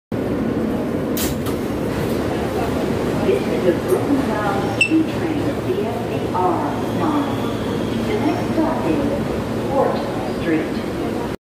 R160 Q train announcement to Brooklyn via the R line.
recorded today.